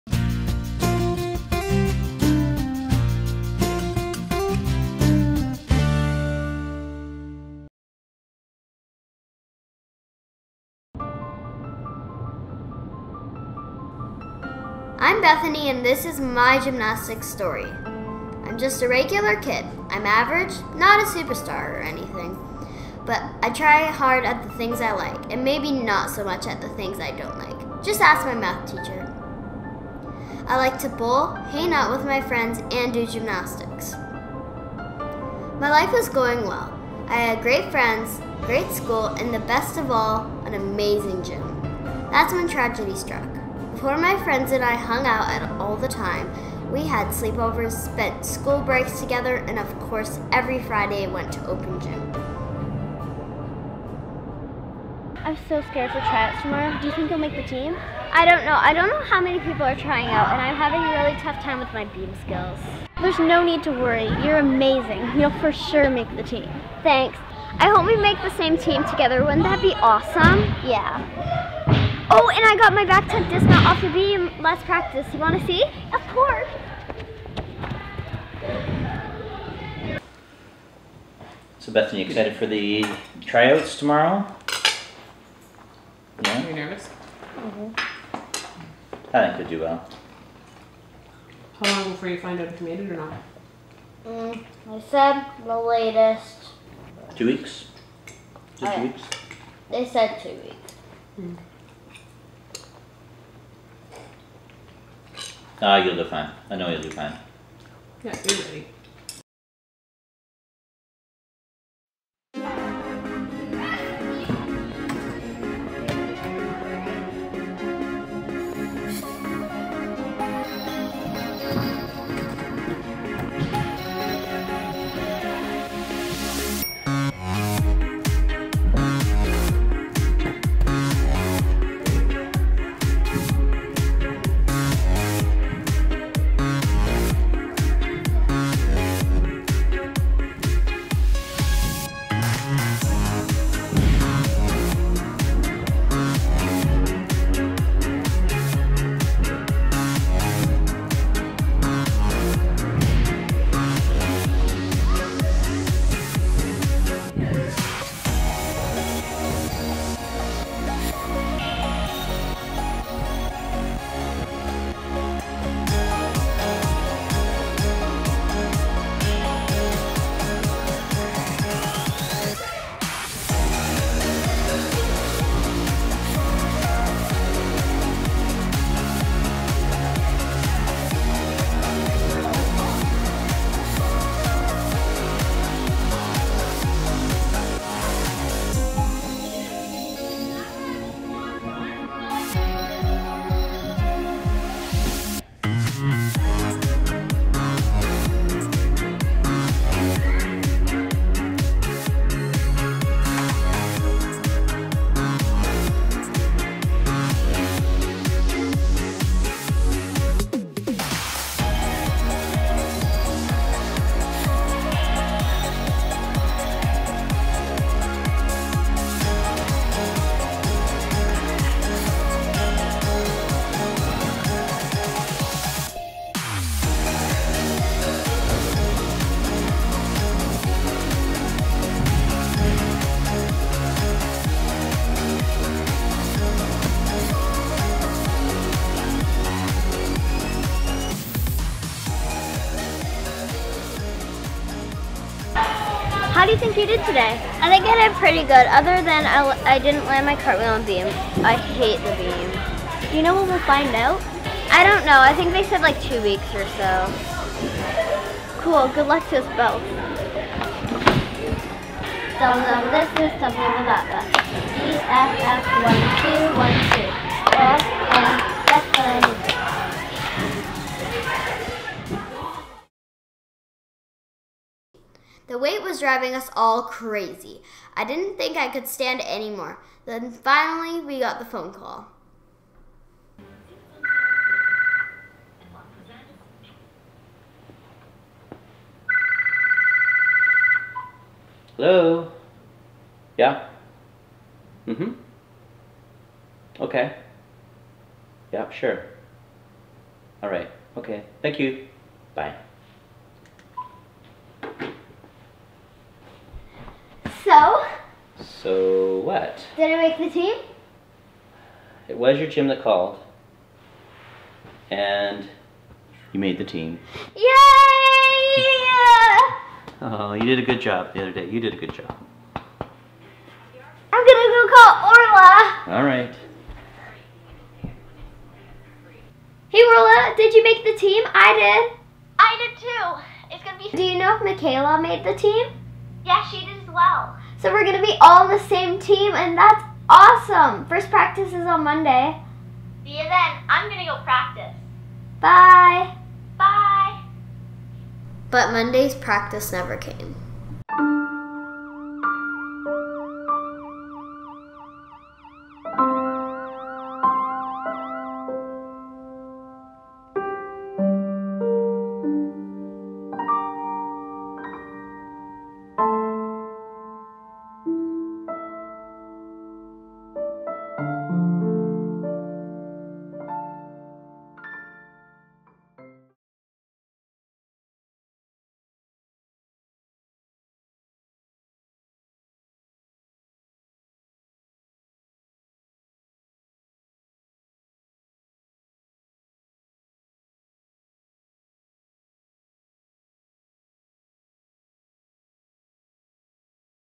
A GYMNAST'S STORY Ep. 1 - Tryouts & Tragedy | Bethany G
Bethany tries out for her local competitive gymnastics team.  Does she make the team?  Tryouts & Tragedy - Episode 1 of A Gymnast’s Story.

A young girl has a great life – great family, great friends, and a great gym.  Then tragedy strikes, forcing them to move.  Bethany is confronted by bullies at her new school and gym.  How is she going to survive?  A Girl's Story is a mini-series about bullying and stuff that happens in life.